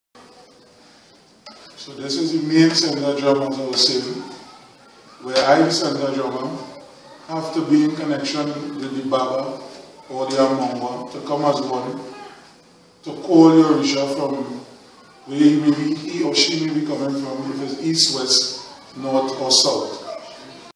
King Rodney
A clip from the first of a series addressing and explaining much of the practices at St. John's Spiritual Baptist Church.